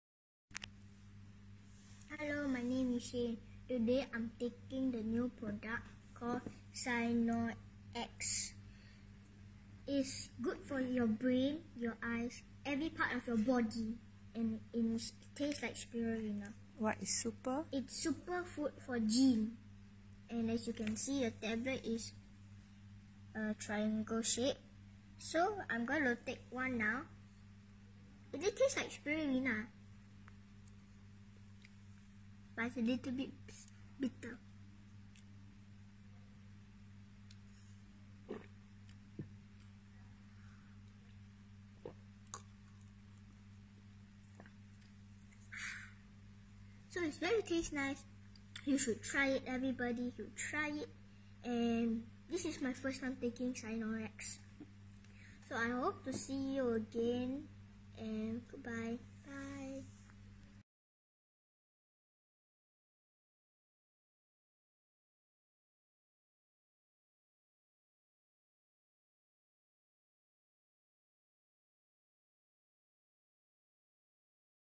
Smart Boy Due To Spirulina, Cyanor X
Customer: Me and my hubby Simon Lim has always a big concern over my son, Shayne Lim, 8years old, health condition because on the day when he was born, the Peadiatrician says the base of his skull was sunken in and he was transferred to a High Dependancy ward in SGH.
I am so glad that Shayne has always been an easy n compliance boy ever since I introduce to him all Elken product and he has been taking regularly too on his own. His reading gene has improved alot ever since he started CyanorX early this year.
Thank you to my company, Elken, for giving us young and old such a wonderful product.